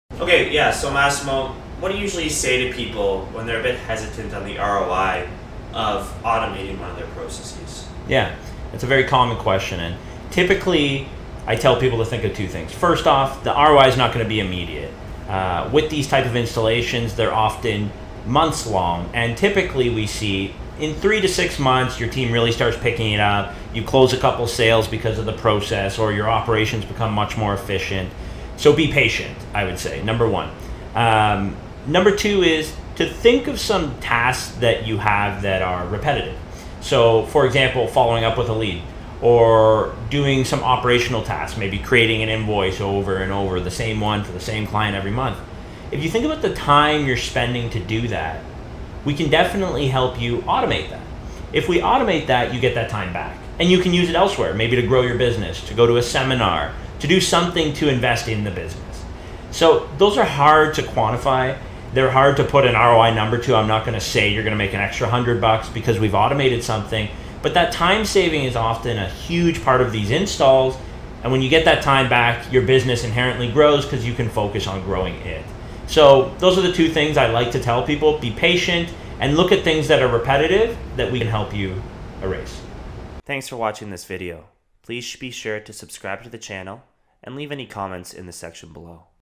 ROI of Automation (2 tips)
If you're considering automating a piece of your business process, then this video is for you. This video lays out 2 essential considerations when planning to automate your business.

If you're considering automating a process in your business then you must first realize that the ROI is not instant. In our experience, it takes anywhere from 3-6 months to realize the full value of automation. The main reason for this is that there is significant transition work that needs to be done from both the process and the people.
The second thing to consider is which processes to automate. If you catch yourself doing something over 3 times per day, then it should have a positive ROI to automate. This could be client communications, following up with leads or even quote preparation.